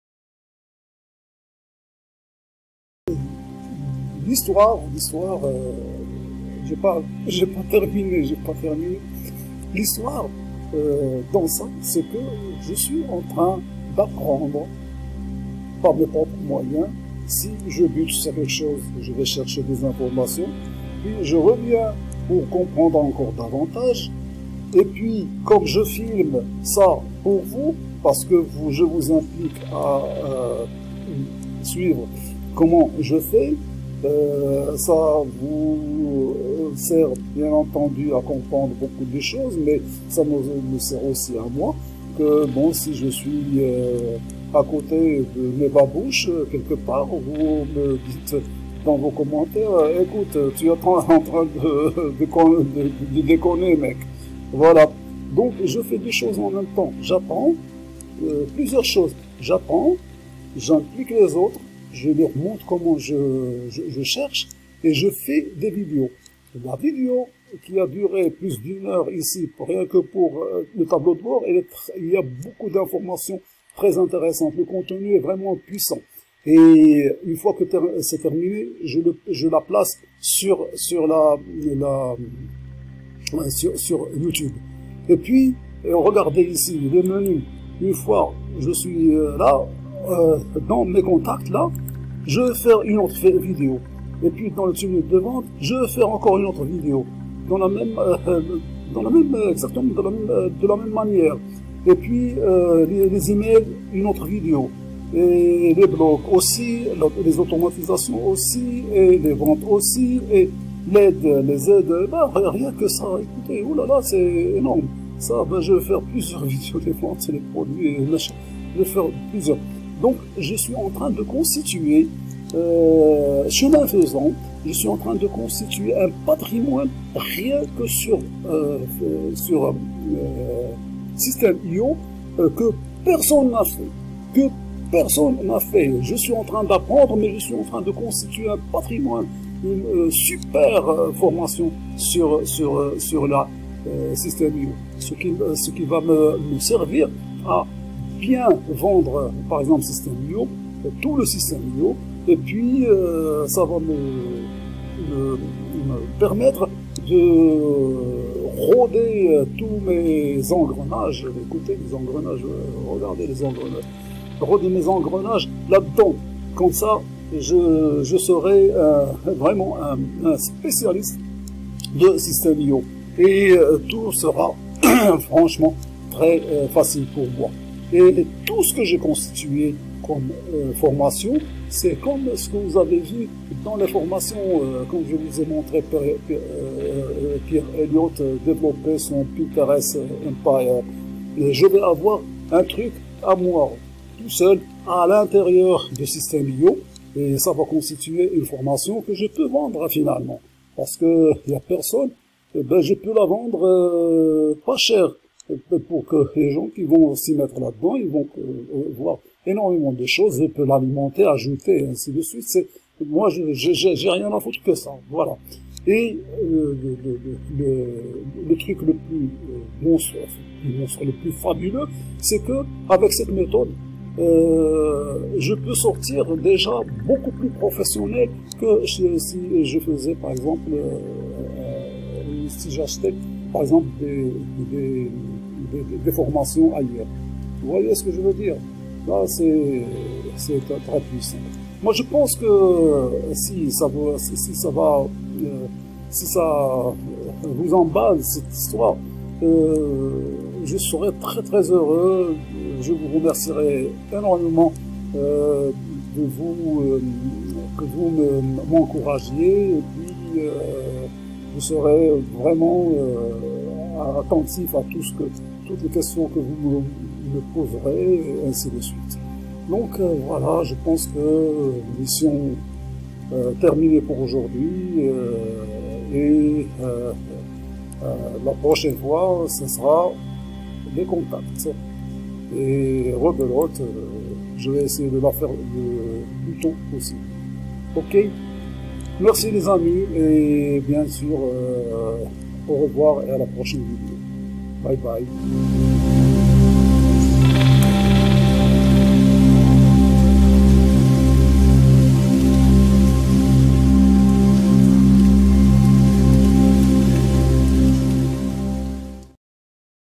COMMENT SYSTEME.IO M'A APPRIS A DEVENIR PROFESSIONNEL EN APPRENANT
Après avoir parcouru Systeme.io superficiellement, j'ai compris que c'est le chemin qu'il faut emprunter avant de m’engager dans toute autre initiative commerciale sur internet.
Voilà ce que j'ai choisi de faire :
Je vais faire comme les autodidactes. Je vais entrer dans systeme.io sans crier garde.
Je vais essayer de comprendre ce qu'il a dans le ventre. Ses postes, ses fonctionnalités, ses astuces, ses interactivités et tout et tout.
Je regarde le tableau de bord
Si je ne comprends pas, je vais pocher le maximum d'informations ailleurs que je résume
Je replonge dans mon tableau de bord pour mieux le comprendre
Je vous implique  dans mon apprentissage
J’en fais une vidéo
Et rebelote pour le sujet d'après puis son suivant etc...
Il va y avoir beaucoup de vidéos
Je vais avoir un patrimoine que personne n'a réalisé autour de Systeme.io.
Du coup, je place cette énorme formation dans mes formations et même la vendre pas chères aux internautes.
Je veux en sortir professionnel

Voici tout ce qui m'attend à regarder er décortiquer :
Le tableau de bord,
Mes formations,
Et la Marketplace.

En sous menus :
Les nouveaux abonnés,
Les paiements,
Les périodes,
Les informations en direct,
Les statistiques générales,
La commission par vendeur,
Les contacts envoyés,
Le total et résumé des commissions,
Les vendeurs affileurs,
La commission des 40% de commissions promise par systeme.io,
Les essais gratuits,
Le nombre des clients,
Le taux de conversion,
Les taux de rétention,
Les offres Marketplace,
Un aperçu de mes formations
Et la grande liste de la Marketplace.
Aujourd'hui j'ai commencé par le tableau de bord, premier terme qui s'annonce dans le menu.